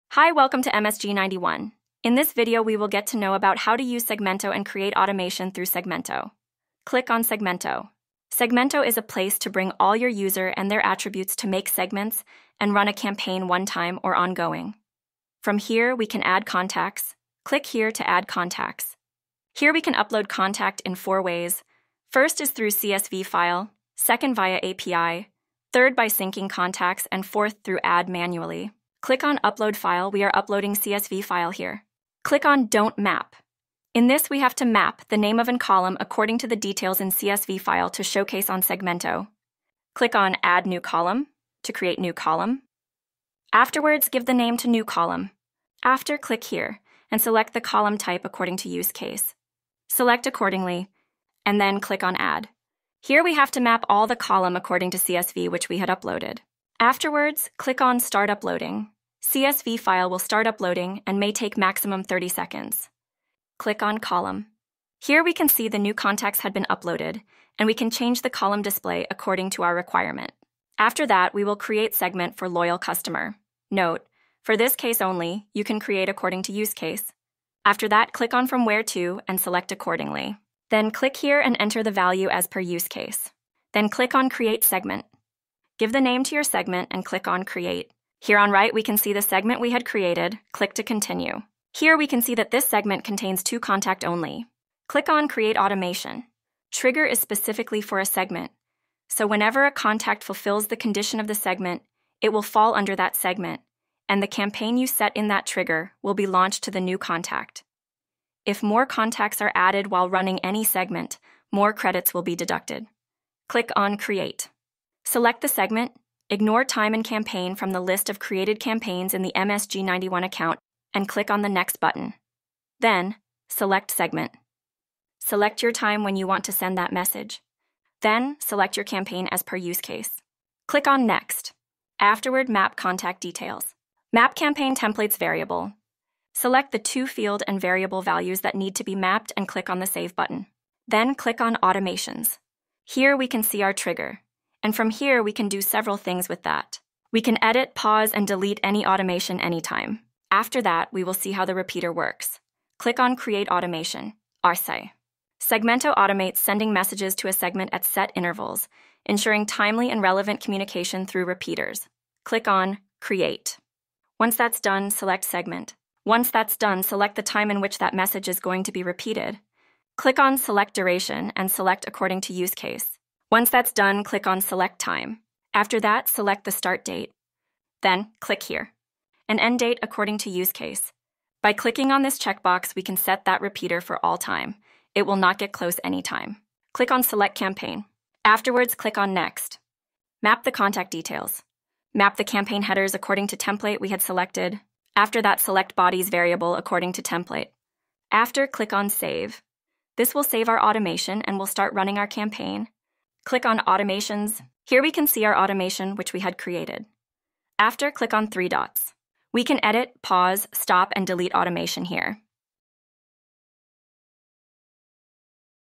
Automate Your Messaging with MSG91 Segmento: Step-by-Step Guide
Learn how to set up automated messaging workflows in MSG91 Segmento. This tutorial covers everything from creating segments to designing and deploying your automation campaigns.